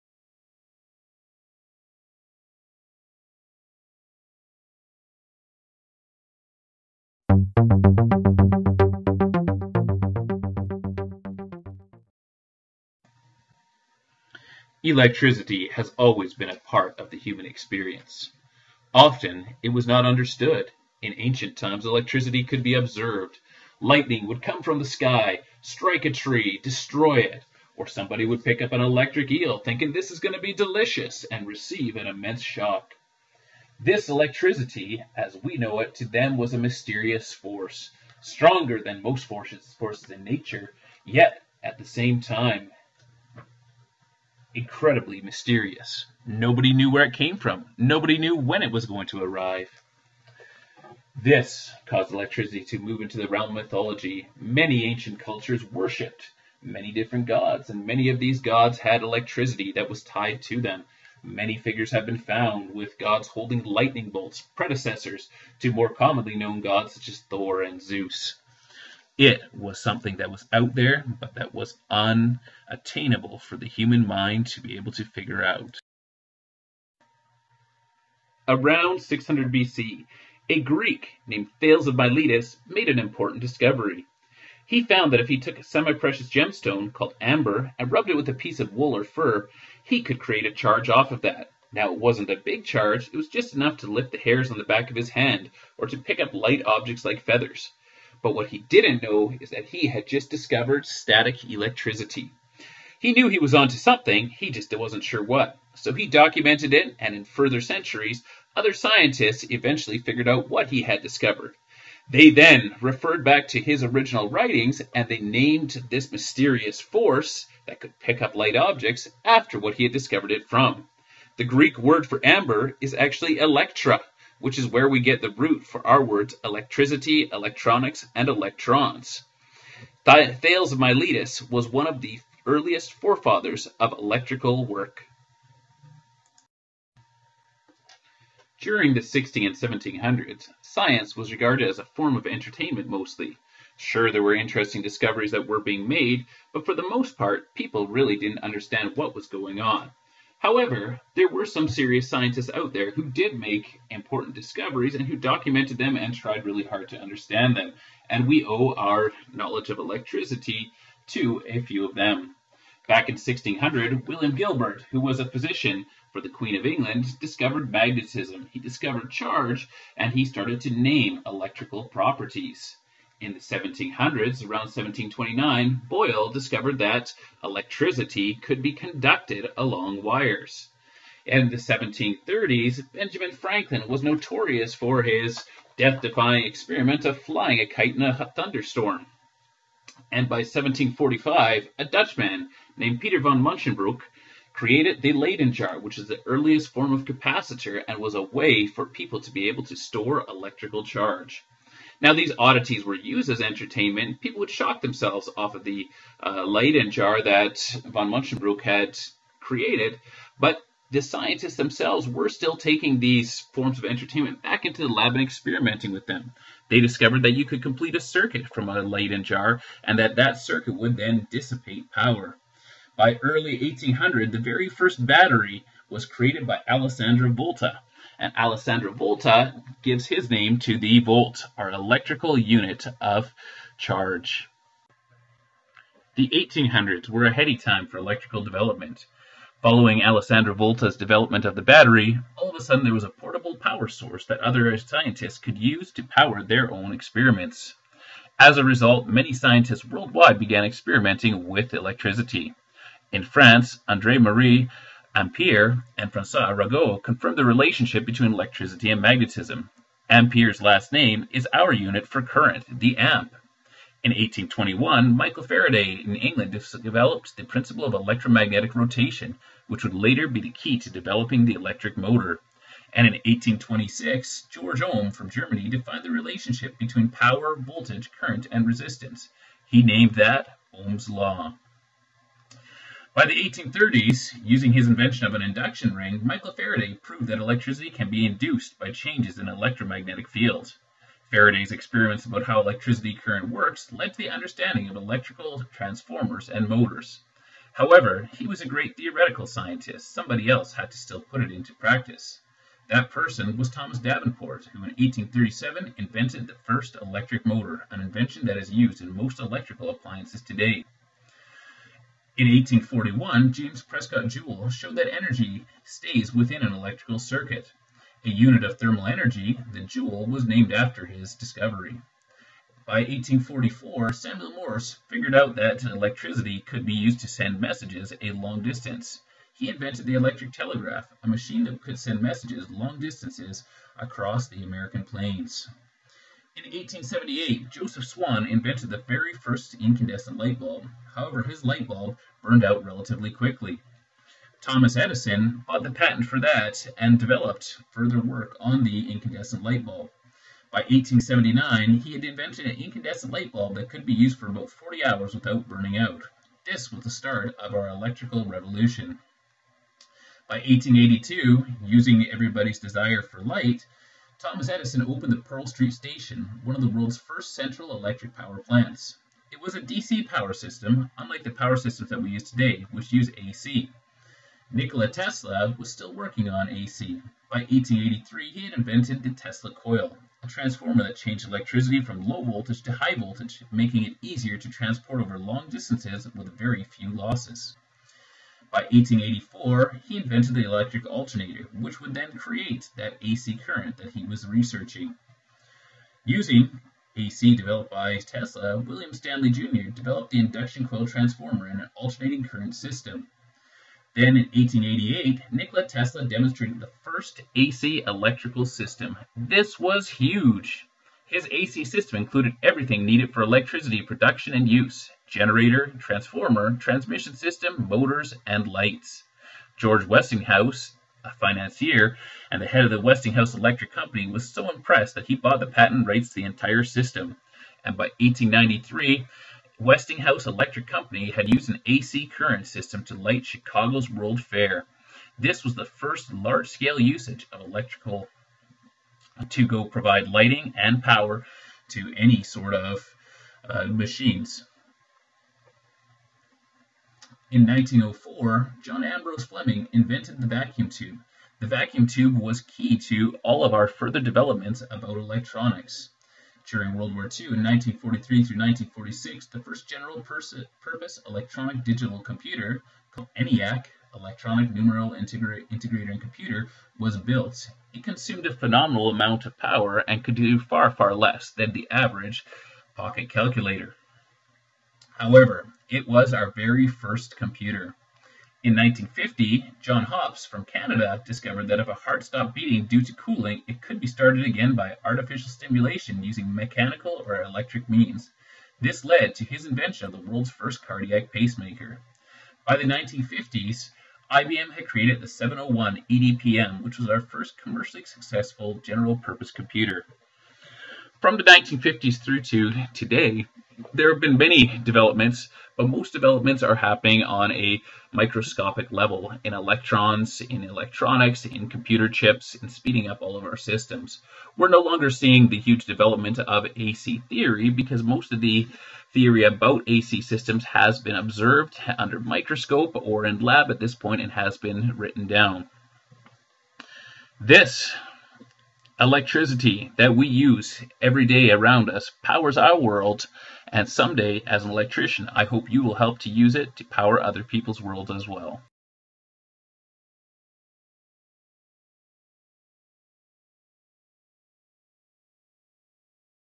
A brief history of electricity
Story about how electricity started and how it evolved over the years